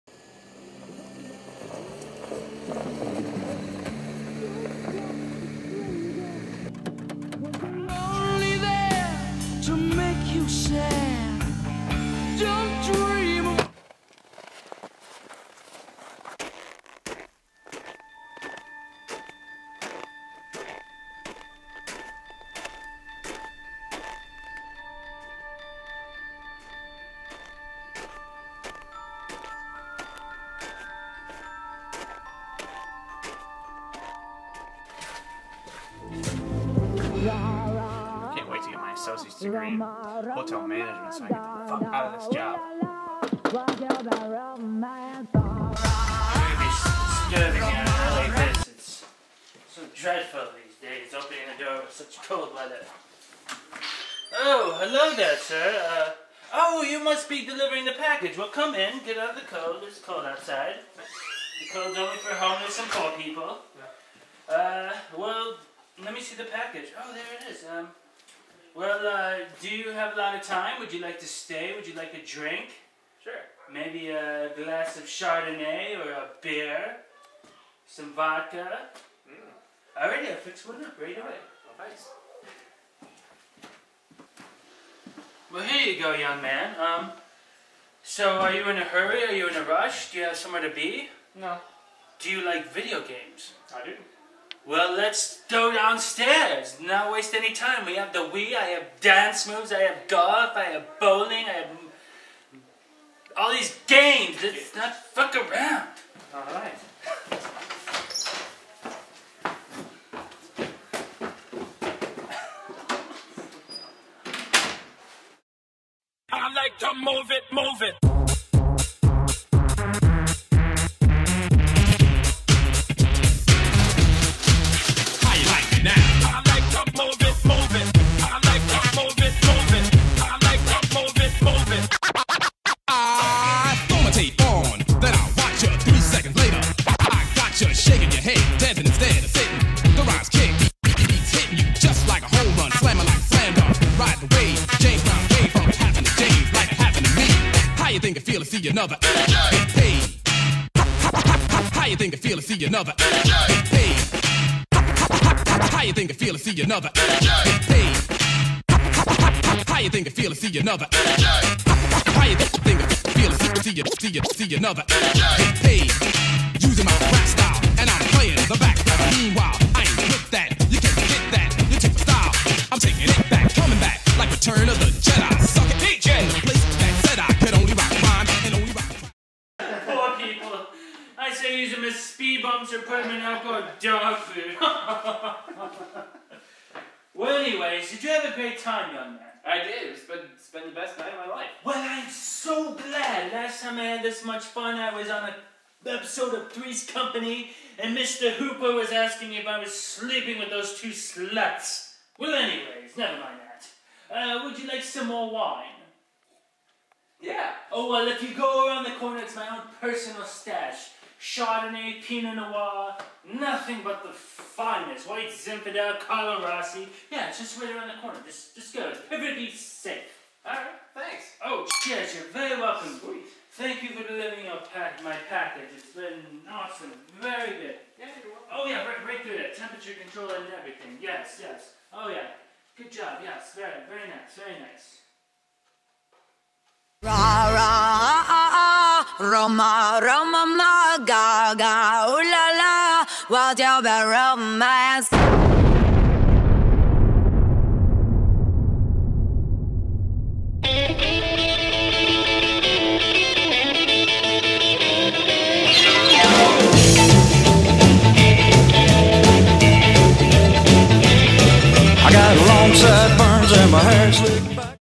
Don't Talk to Strangers!
Tony Strong and I shot this movie a few days before christmas, his brother chuck operated the camera. I know its not awesome but it was fun to shoot something!